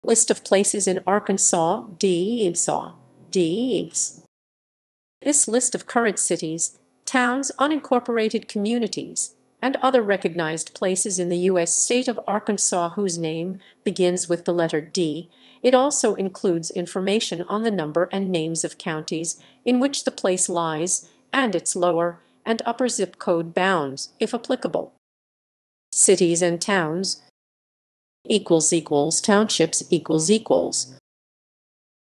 List of places in Arkansas: D
This list of current cities, towns, unincorporated communities, and other recognized places in the U.S. state of Arkansas whose name begins with the letter D. It also includes information on the number and names of counties in which the place lies, and its lower and upper zip code bounds, if applicable.